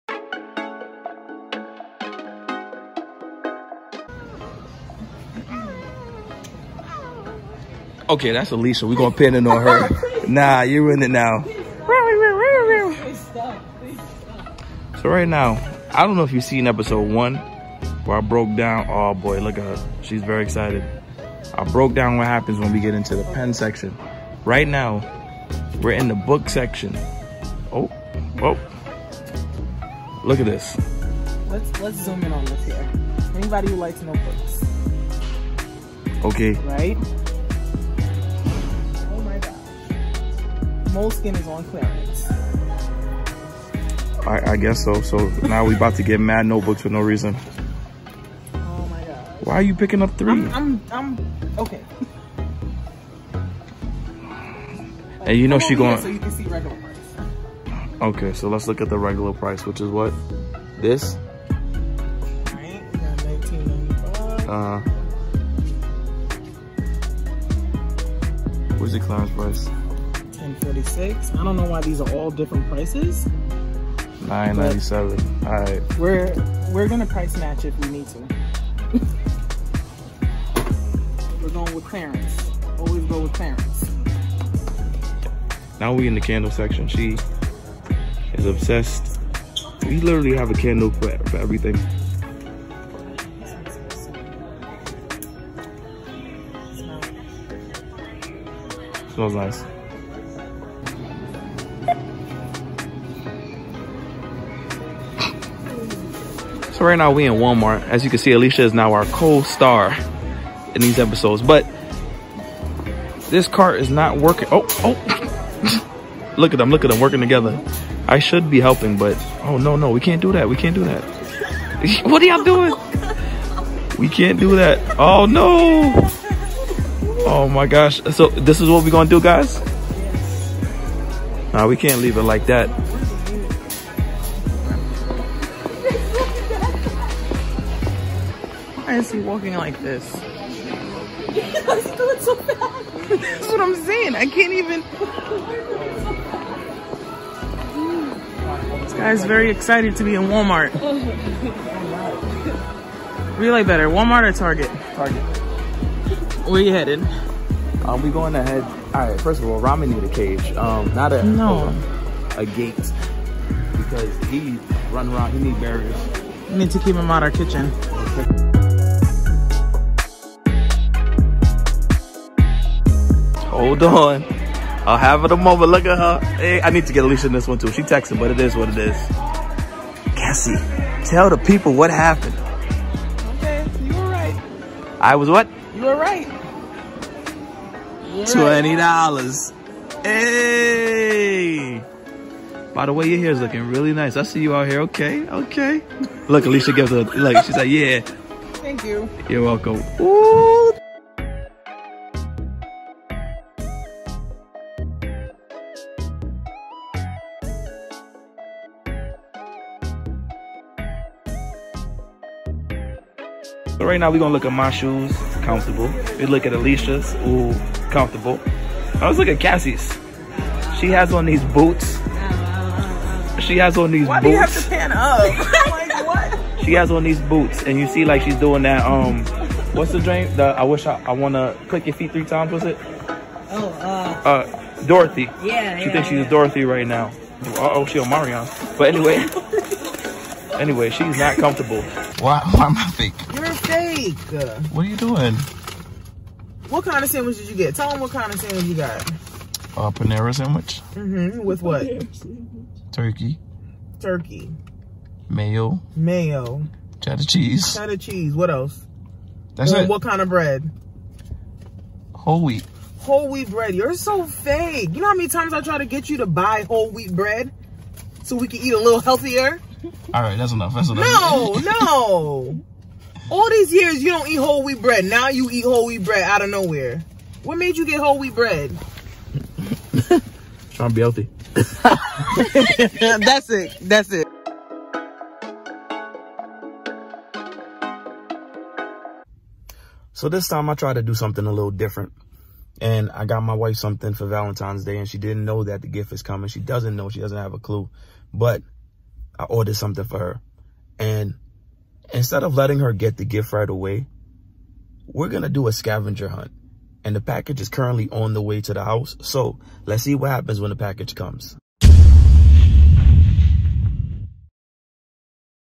Why does my wife think I’m fake?? 😳😳😳 (Vlog Ep3)
Vlog episode 3 ( Denzel, Cassie, and Alicia go on a shopping adventure, and Denzel has plans for Valentine’s Day 👀)